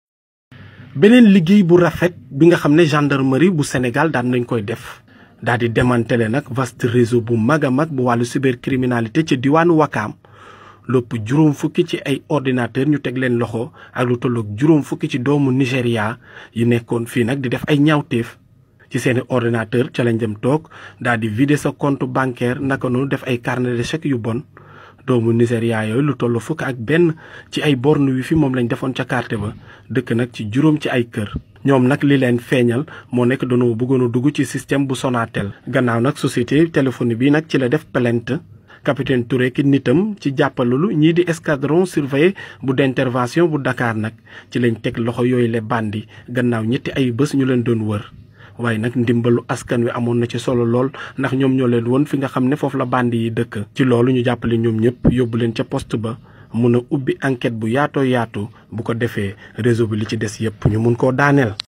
Urgent...la brigarde cybercriminalité a Ouakam
La brigarde de la gendarmerie arrêt une cinquantaine de Nigériens a Ouakam avec des ordinateurs des portables et de l'argent volé.
Cybercriminalité au Sénégal